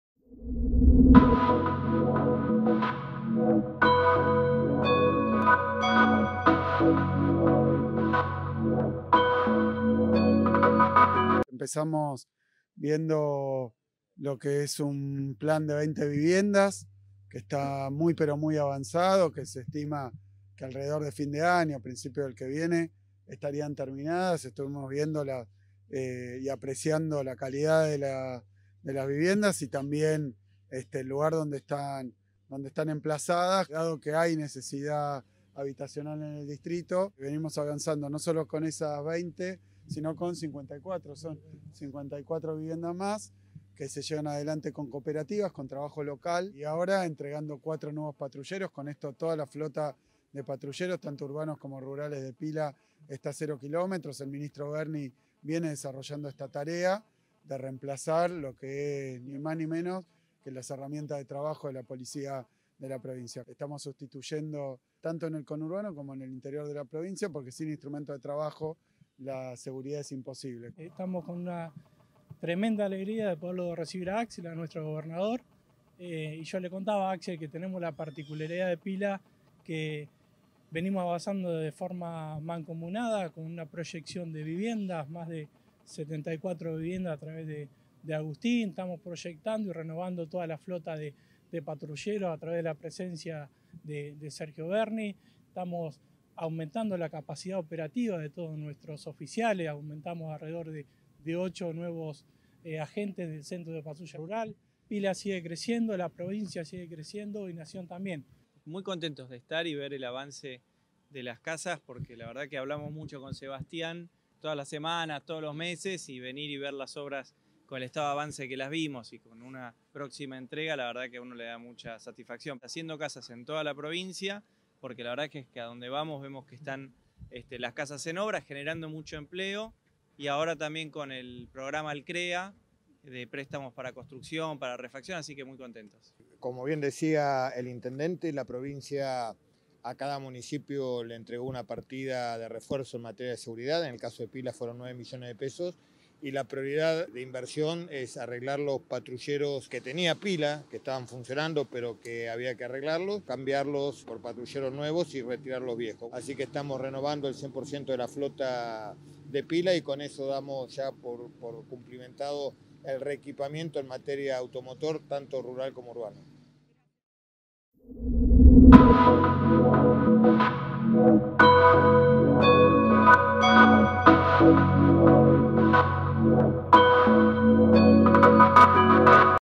Kicillof recorrió los avances de las obras de construcción de viviendas en Pila
Además, el Gobernador entregó cuatro patrulleros y visitó emprendimientos productivos y culturales.

El gobernador de la provincia de Buenos Aires, Axel Kicillof, recorrió esta tarde los avances de las obras de construcción de 20 viviendas en el municipio de Pila, donde visitó también emprendimientos productivos y encabezó la puesta en funcionamiento de cuatro nuevos patrulleros. Fue junto a los ministros de Hábitat y Desarrollo Urbano, Agustín Simone; de Producción, Ciencia e Innovación Tecnológica, Augusto Costa; y de Seguridad, Sergio Berni; y el intendente interino, Sebastián Walker.

En ese marco, Kicillof destacó que “la construcción de estas viviendas es realizada por cooperativas, con trabajo local, a partir de la articulación entre la Provincia y el municipio”. “Esta es una modalidad que nos permite no solo generar soluciones habitacionales para los y las bonaerenses, sino también promover la creación de empleo en cada uno de los partidos”, aseguró.

Las 20 viviendas son construidas por el Ministerio de Hábitat y Desarrollo Urbano, a partir de una inversión de 110 millones de pesos. Están ubicadas sobre la calle Manuel Morillo e integran la primera etapa de un proyecto de 74 casas; en tanto que ya está en marcha una segunda fase para otras 54 soluciones habitacionales en el barrio.

“Para nosotros es una satisfacción muy grande visitar cada municipio y ver los avances de las obras de viviendas, más aún cuando, como en este caso, están próximas a entregarse”, explicó Simone, al tiempo que subrayó que “tanto con la construcción de casas como con el lanzamiento del programa de créditos Buenos Aires CREA, estamos contribuyendo con la creación de empleo local”.

El Gobernador también encabezó la puesta en funcionamiento de cuatro nuevos patrulleros, que se suman a los tres móviles que el Ministerio de Seguridad bonaerense había entregado en mayo para equipar al cuerpo de Policía Rural. “Con esta entrega de móviles, hemos renovado toda la flota de patrulleros rurales y urbanos de Pila con rodados cero kilómetro. Este es un trabajo que estamos realizando tanto en el conurbano como en el interior de la Provincia, ya que es imposible brindar más seguridad si nuestra fuerza no cuenta con los instrumentos necesarios”, resaltó Kicillof.

Por su parte, Berni señaló que “la Provincia le entregó a cada municipio una partida de refuerzo en materia de seguridad, que en el caso del municipio de Pila ascendió a los 9 millones de pesos”. “Nuestra prioridad era arreglar los patrulleros que ya tenía el distrito y cambiarlos luego por patrulleros nuevos: hemos renovado el 100% de la flota, avanzando con el reequipamiento automotor tanto de móviles rurales como urbanos”, indicó.

En tanto, el intendente Walker remarcó: “Gracias al trabajo mancomunado entre Municipio, Provincia y Nación, Pila viene avanzando en materia de seguridad y soluciones habitacionales”. “Ya hemos renovado toda la flota de patrulleros, aumentando la capacidad operativa de la Patrulla Rural, y estamos construyendo más de 74 viviendas”, dijo.

Asimismo, las autoridades se acercaron hasta el predio en el que se construye la Casa de la Provincia, ámbito que agrupará las distintas dependencias de la administración pública con el objeto de facilitar trámites y consultas.

Recorrida por emprendimientos culturales y productivos

En su visita al municipio de Pila, Kicillof recorrió también el Cine Teatro Municipal “Padre Adolfo Anastasio”, que cuenta con una sala de 178 butacas; agenda de actividades con cartelera comercial y estrenos; y obras de teatro gratuitas. Entre el 25 y el 28 de agosto, allí se llevarán a cabo la primera edición del Festival Audiovisual Pila y la Competencia de Fotografía Local y Cortometrajes Internacionales.

Por último, Kicillof afirmó que “estamos encarando el proyecto del frigorífico en la ciudad que pronto tendrá su llamado a licitación, para que aquí también se le pueda agregar valor a la producción ganadera”. “Todos los aspectos de la vida cotidiana de los vecinos y vecinas de Pila cuentan con la presencia de un Estado transparente y que brinda resultados”, concluyó.